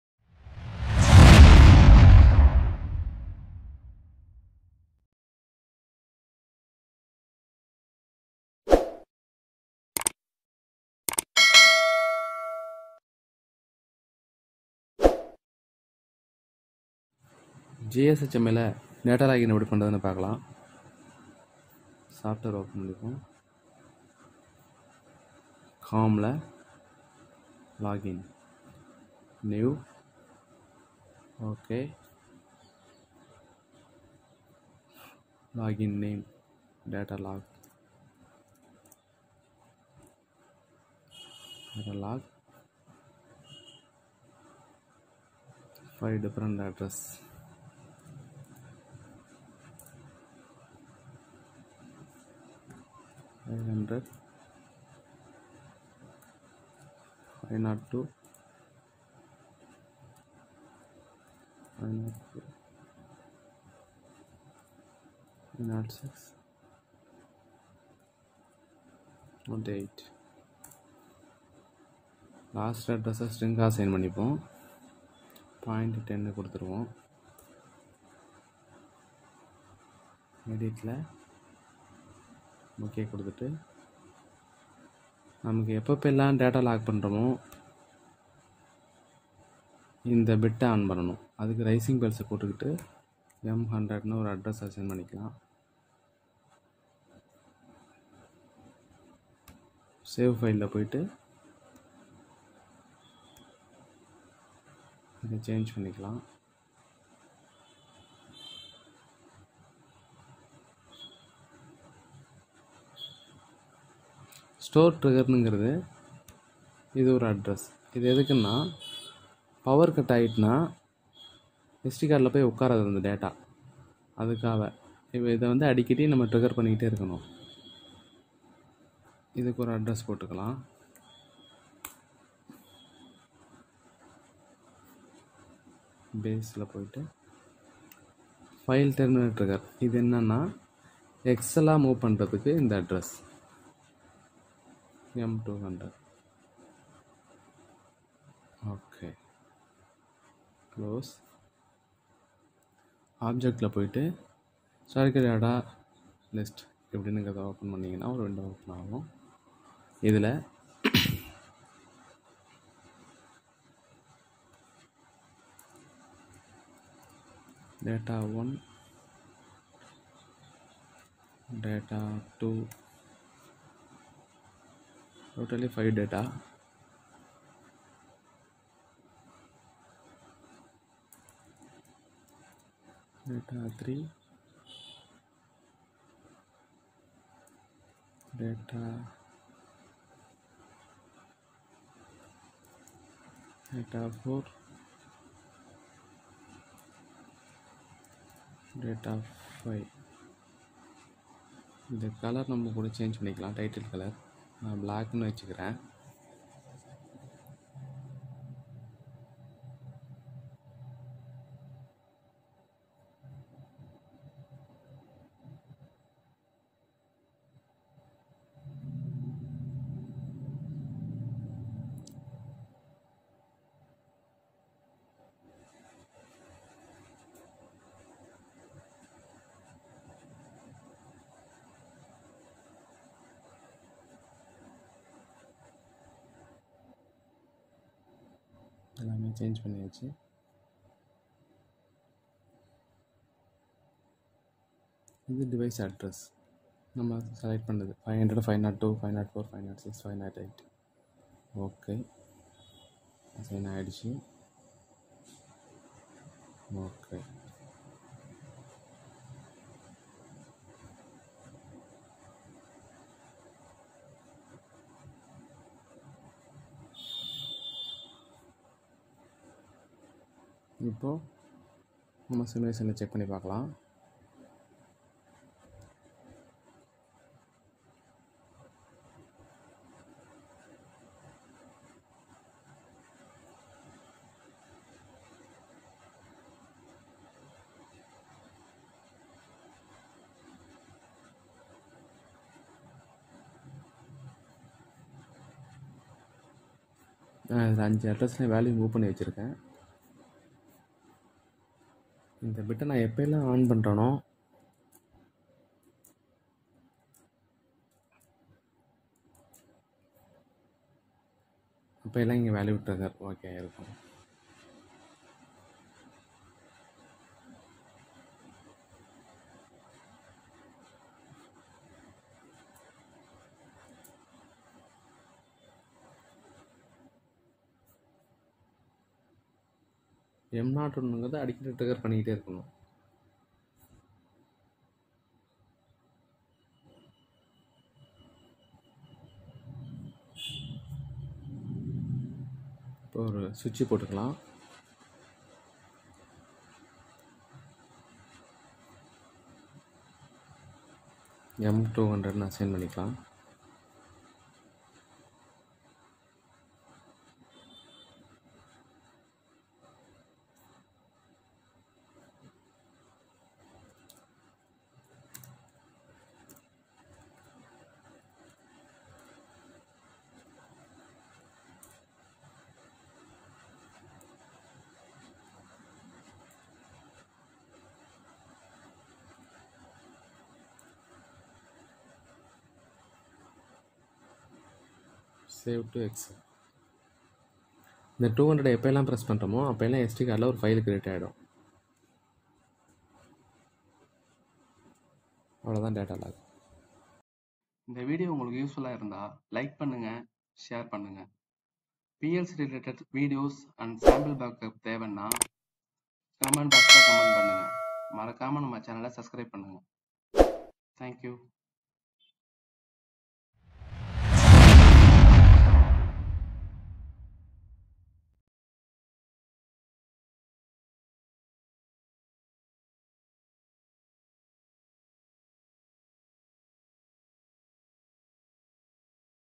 How to Create Datalog in GS-HMI with Simulation||Sample Video
Creation of Datalog in GS-HMI with Simulation Using GT-Designer 3 Software.
Please Follow our Channel for more contents related to Mitsubishi FA Programming